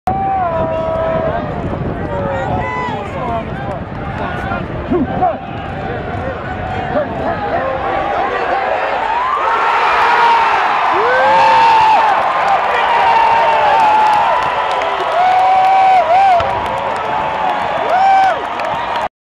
DJ Harper hauls in a 21 yard TD pass from Joe Southwick Boise State vs MiamiOhio 9 15 2012 036
Scoring triple play, Boise State gets first offensive touchdown of season, Joe Southwick gets his first touchdown pass of season, and DJ Harper gets his first of 4 touchdowns on the day